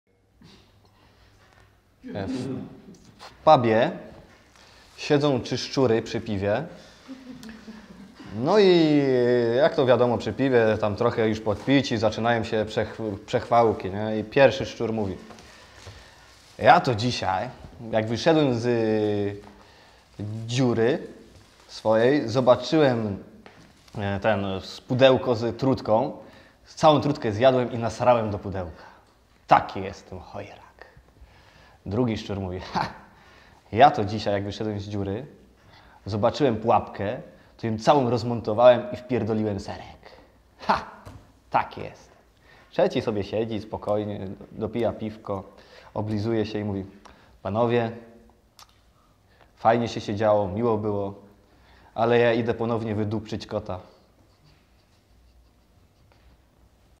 Wombat z Sucharem: Trzy szczury na piwie
PEGI: Wulgaryzmy

Treść suchara:
Siedzą w pubie trzy szczury i nieźle tankują. Po paru browarach
szczury zaczynają się miedzy sobą chwalić jacy są mocni i odważni.
Normalna pijacka napinka. Szczur numer 1 mówi "jak dzisiaj wylazłem z
dziury zobaczyłem pudełko trucizny. Zjadłem wszystko, nawaliłem do
pudelka i poszłem spać". Szur numer 2 "a ja jak wylazłem z dziury
zobaczyłem pułapkę. Spokojnie ja rozmontowałem, zjadłem serek i
poszłem spać". Szczur numer 3 siedzi, myśli, przechyli kufel piwa,
oblizał się i powiedział: "Panowie życzę milej zabawy ale ja idę
jeszcze raz wydupczyć kota!"